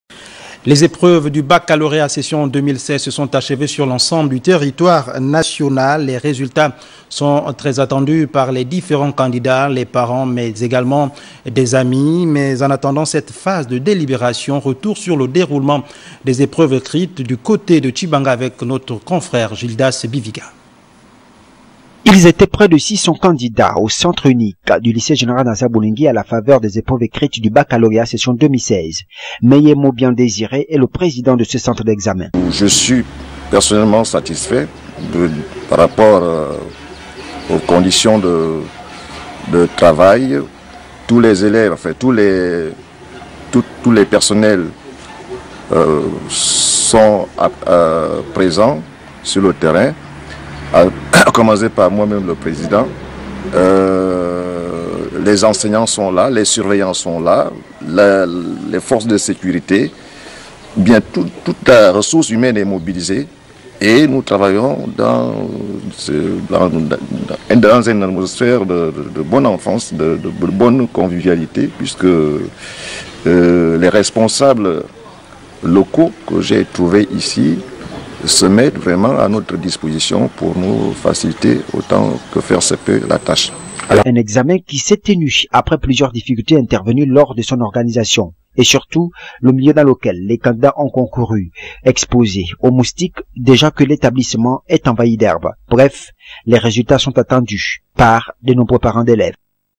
RTG/Retour sur le déroulement des épreuves du Baccalauréat session 2016
Monsieur Meye M'obiang Désiré donne son avis sur le déroulement des épreuves du baccalauréat session 2016